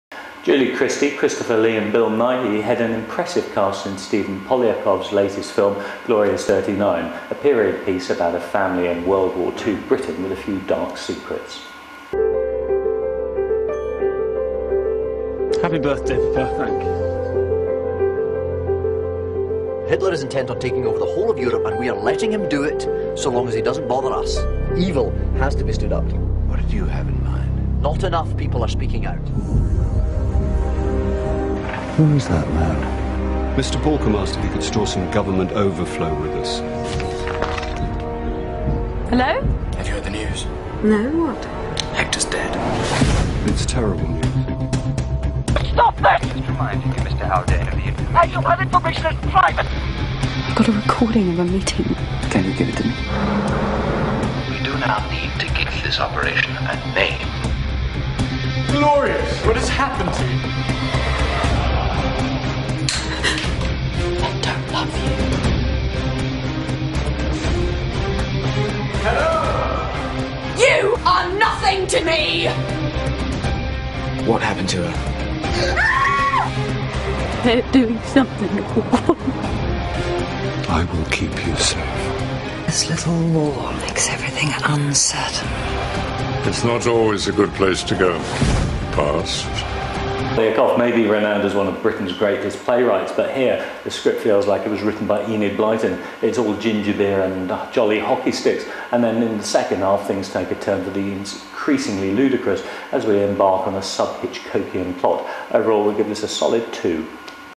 Glorious 39 film review
Daily Mirror film critic David Edwards reviews the film Glorious 39